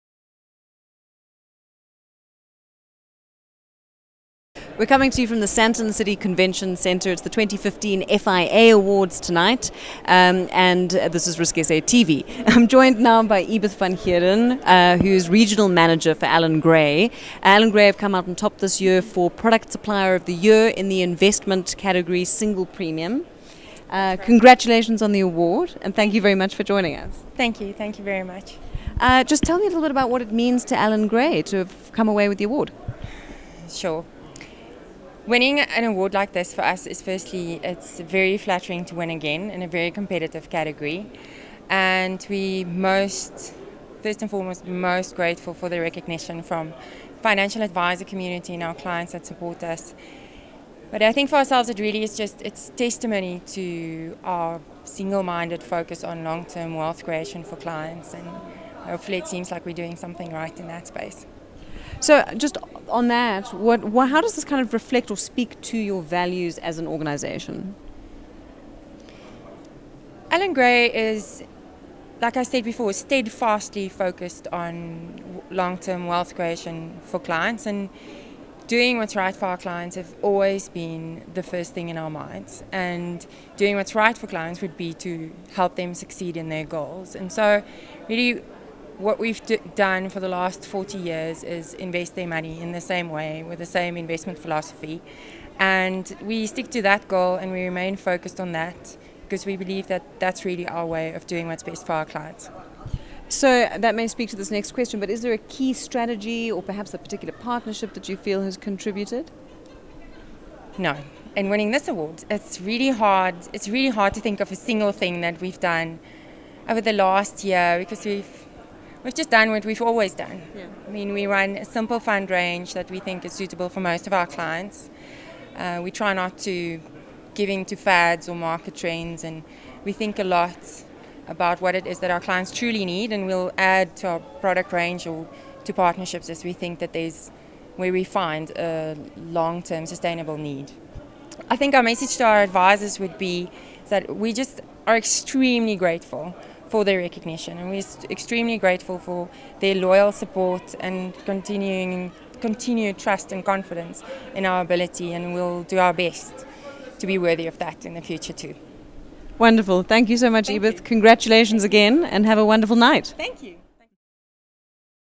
FIA AWARDS 2015: Ebeth van Heerden, regional manager at Allan Gray
RISKSA TV News reports from the FIA Awards 2015 at the Sandton Convention Centre. We speak to Ebeth van Heerden, regional manager at Allan Gray. Allan Gray won the award for Product Supplier of the Year in the Investment category: Single Premium.